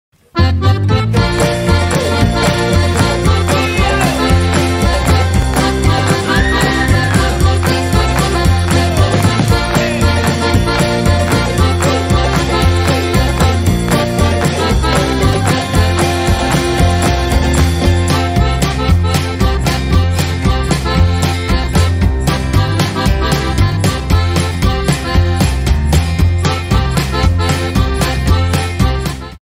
Snorty's Term-time Weekdays at Hatton!
Snorty is BACK! Join Snorty and his friends on term-time weekdays for a day of ANIMALS, ACTION & ADVENTURE! Plus, join Snorty twice a day for a fun farmyard story and sing-a-long session. Running until 8th April 2022, book your tickets at adventure.hattonworld.com